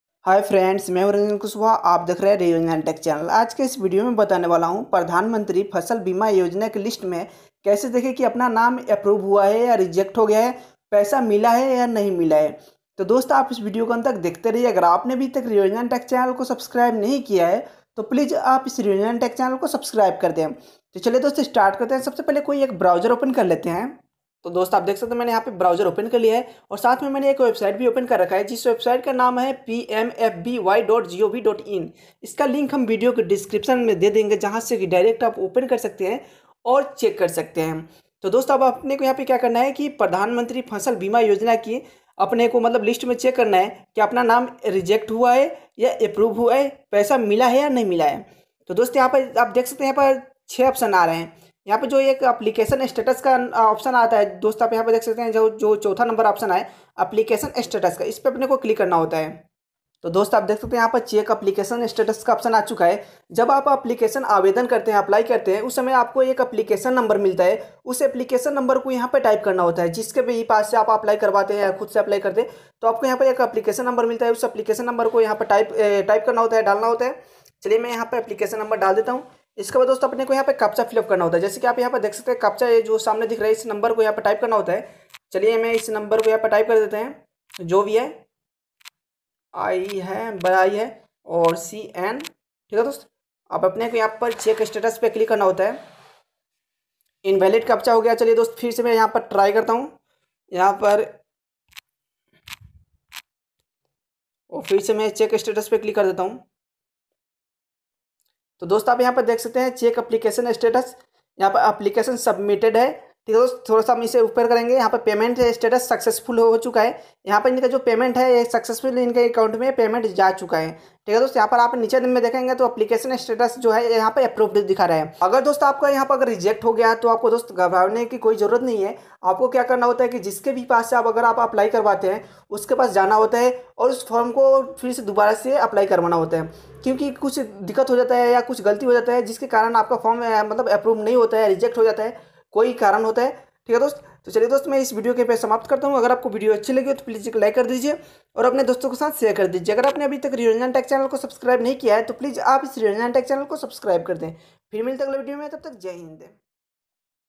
प्रधानमंत्री फसल बीमा योजना लिस्ट 2021 - Pradhanmantri Fasal Bima Yojana list - PMFBY List
Pradhan Mantri Fasal Bima Yojana list 2021, PMFBY list me kaise dekhte hai. Ki application approve huya ya reject paisa Mila ya nahi. Yehi es video me bataya hai agar video pasand aaye to video link kare.
PM Yojana list 2021,
How to check Pradhanmantri Fasal Bima Yojana list. sarkari Yojana ki Jankari